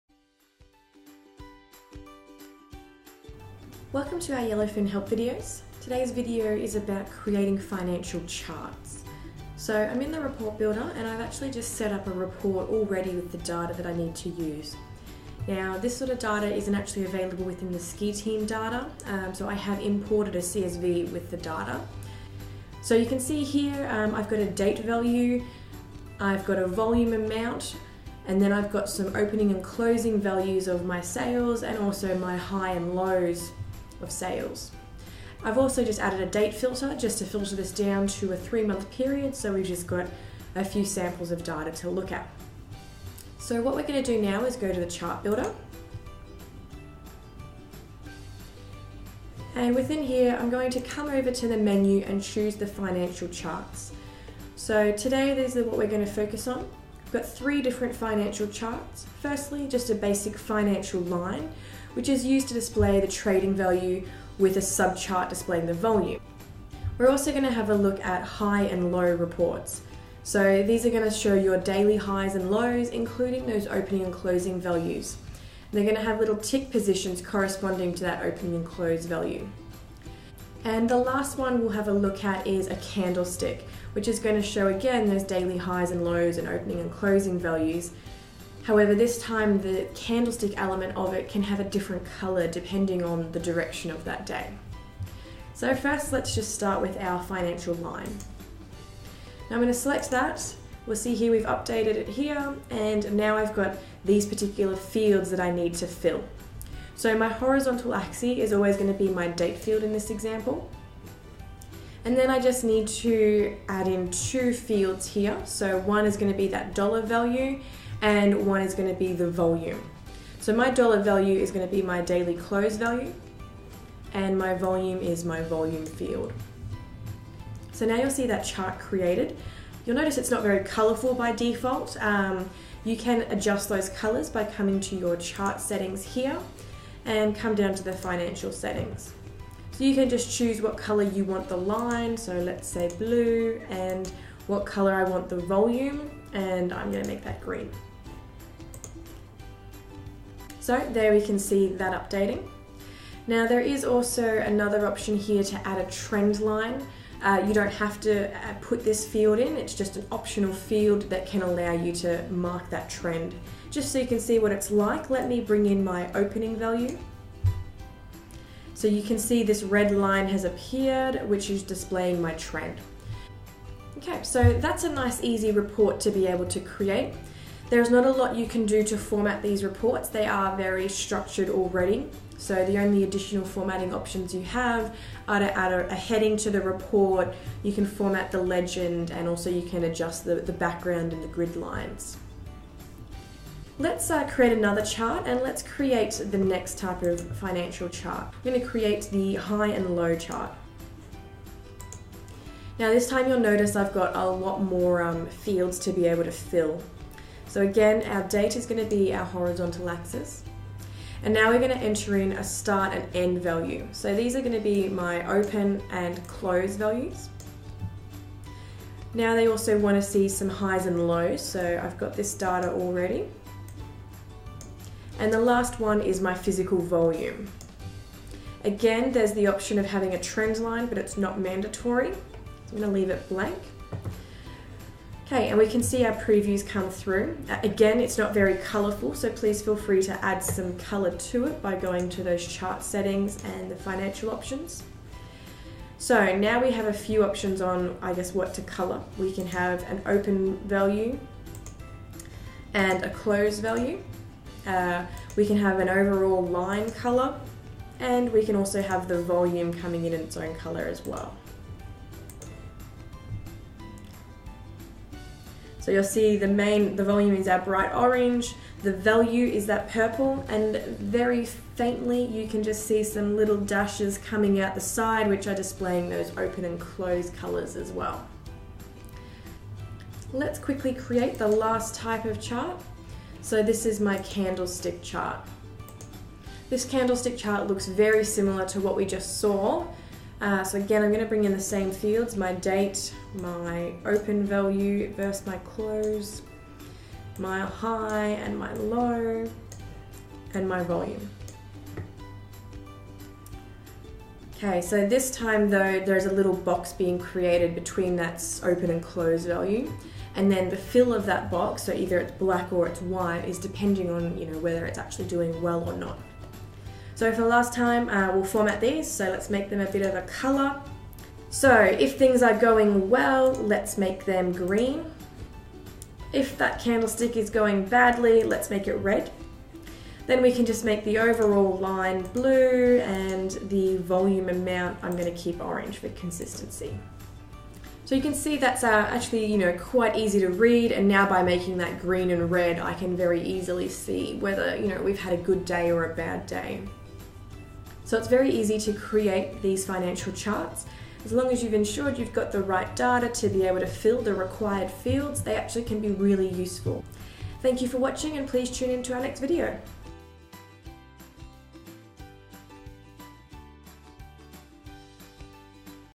Creating Financial Charts
An official Yellowfin Help video explaining how to create Financial Charts including Financial Line charts, High Low charts and Candlestick charts.